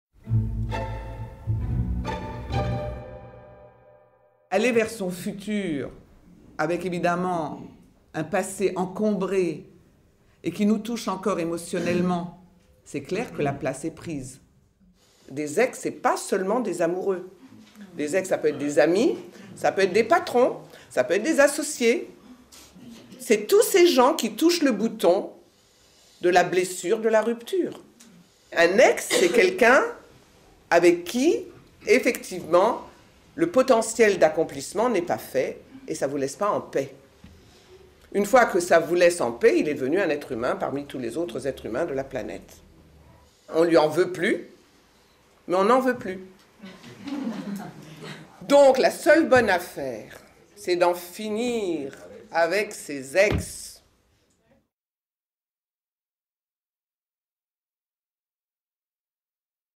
En finir avec nos "Ex"
NOUVEAU : Découvrez le coffret "52 cartes pour vivre l'amour" par Arouna Lipschitz dès maintenant

Avez-vous vraiment fait le deuil de vos "Ex" ?
Des années après une rupture certains continuent à regretter que ça n'ait pas marché. D'autres répètent indéfiniment le même schéma amoureux parce qu'ils n'ont pas tiré les leçons de leurs échecs...
Un pack pour faire le point : comprendre comment et pourquoi il est important de bien en finir avec ses  "Ex"